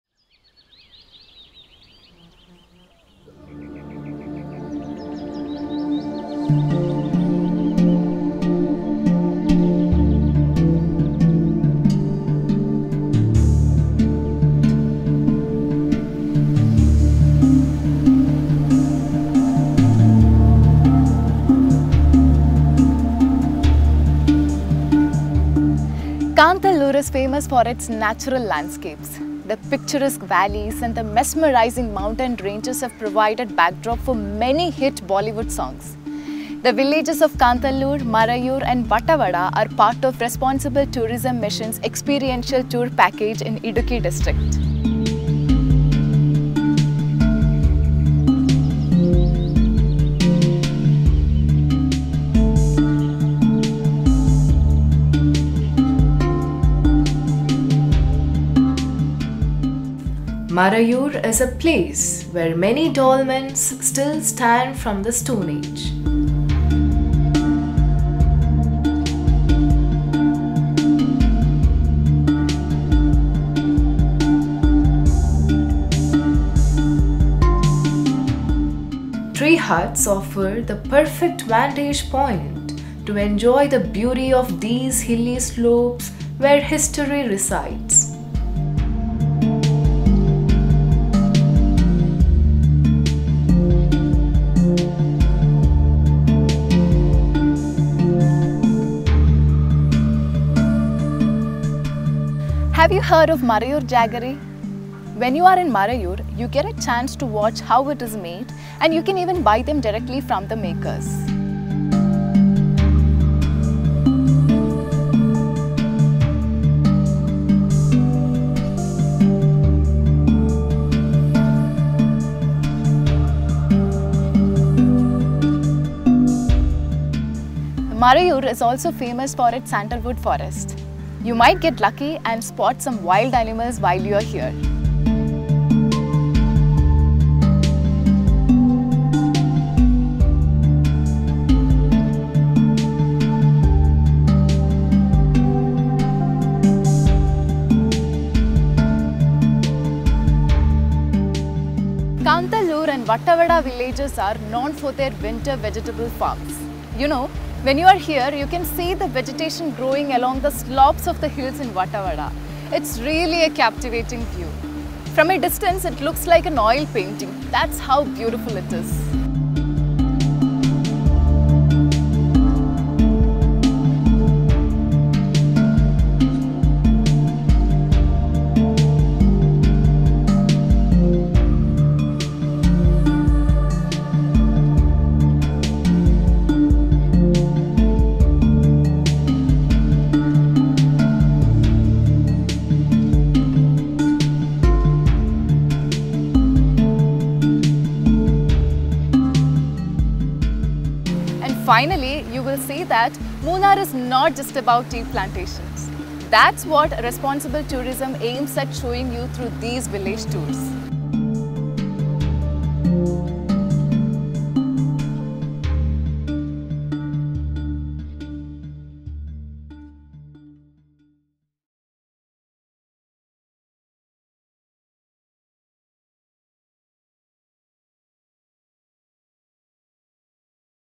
Idukki Experiential Tour Package | Kanthalloor | Marayoor and Vattavada |Kerala Responsible Tourism
The Responsible Tourism package in Munnar includes the villages of Kanthaloor, Marayur and Vattavada. Kanthalloor and Vattavada are popular for vegetable farms. Marayur is a place where many dolmens still stand from the Stone Age.